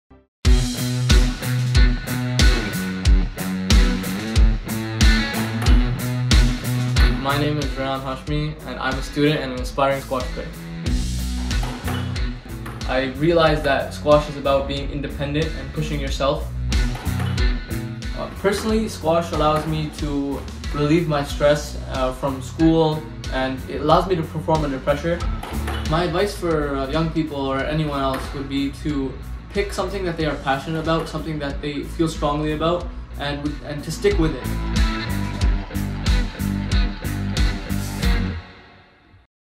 The Rise of a Squash World Champion | Interview Teaser | Young Pakistani Squash Player | BPozitive
Welcome to BPozitive! This is an Interview teaser of a Future World Champion Rayan Hashmi!

BPOZITIVE is a platform and a beacon of hope and inspiration to transform your emotional weaknesses into a quantum's of positive strengths.